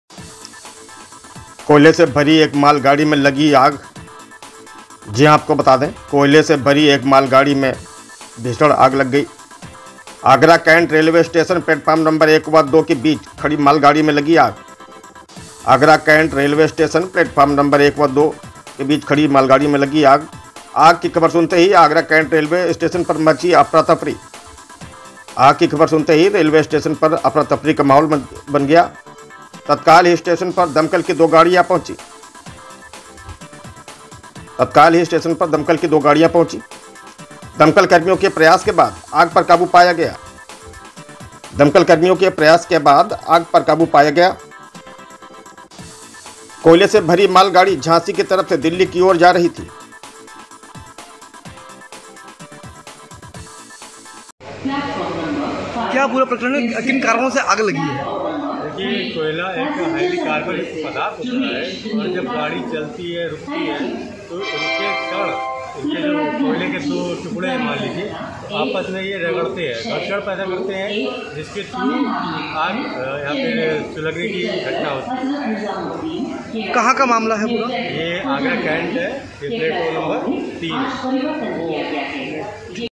आगरा: कोयले से लदी मालगाड़ी में लगी आग
आगरा: कैंट स्टेशन में कोयले से लदी मालगाड़ी में लगी आग, दमकल कर्मियों ने बुझाई आग
 @News ST